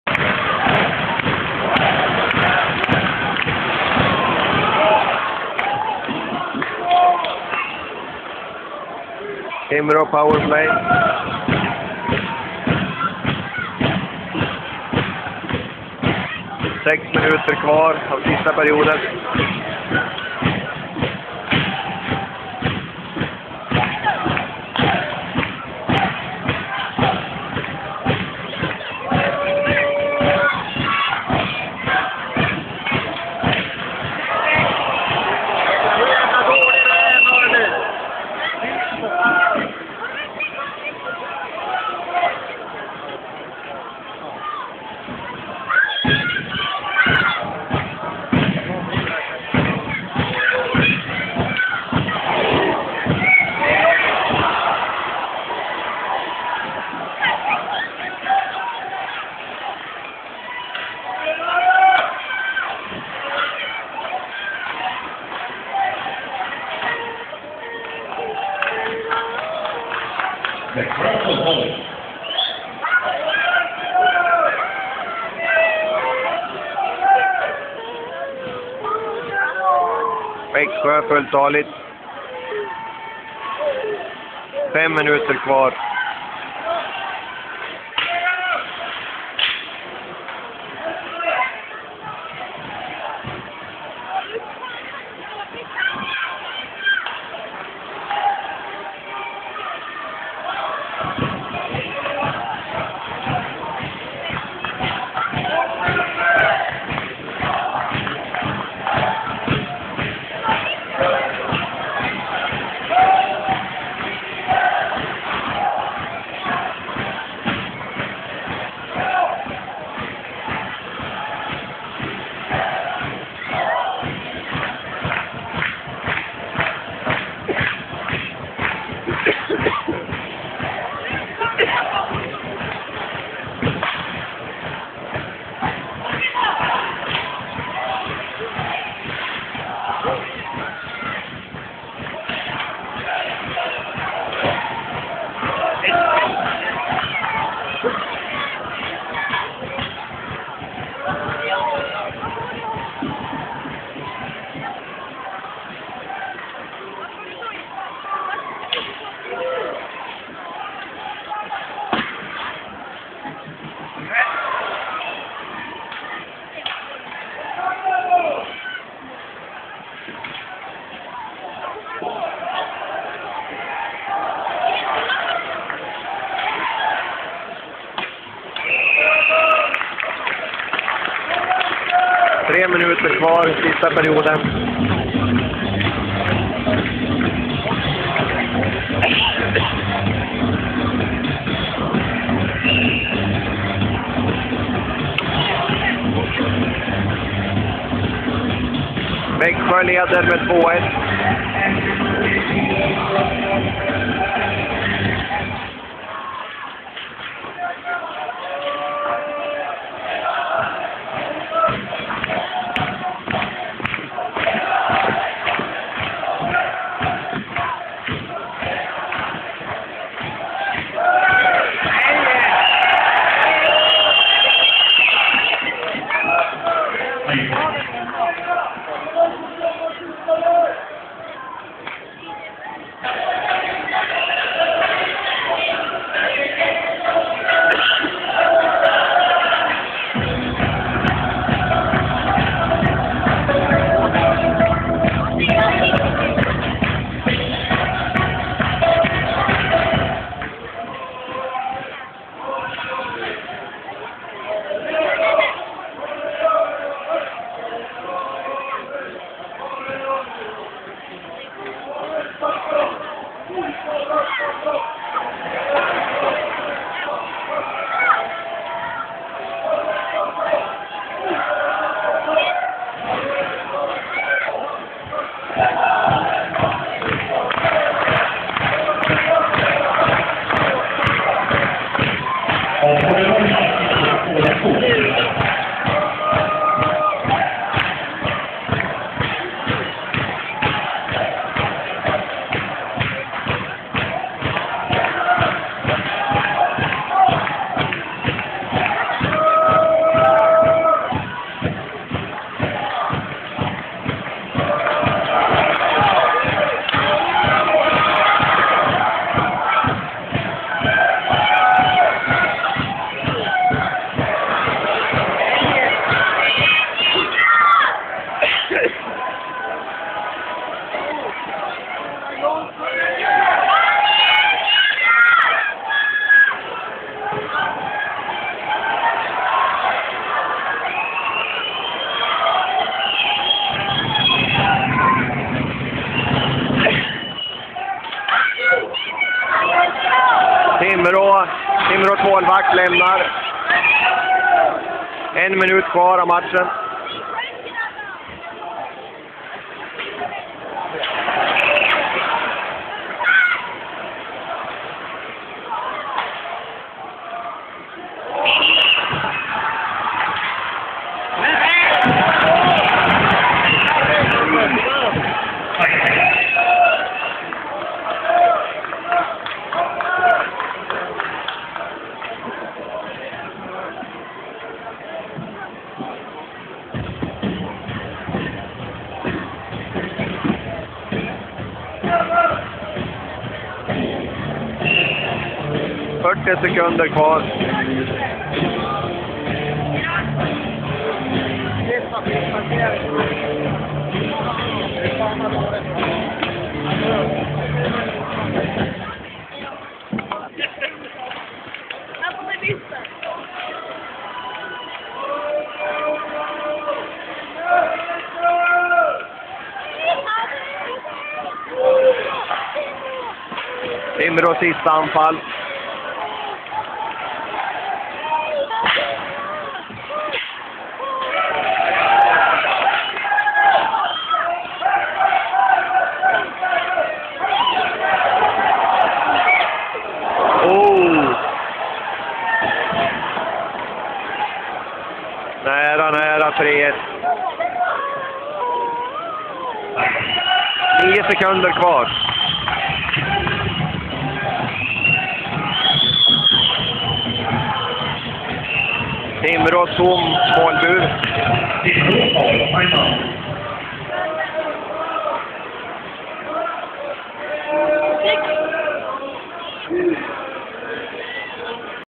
Timrå IK - Växjö Lakers 2011
Timrå IK plays against Växjö Lakers, Sunday 11 dec. -2011 in Timrå. Växjö leads with 2-1 and also won the match with that score. This video is from the third period...at the end.

This video is from my mobile phone (a Sony/Ericsson).

Ulf sawert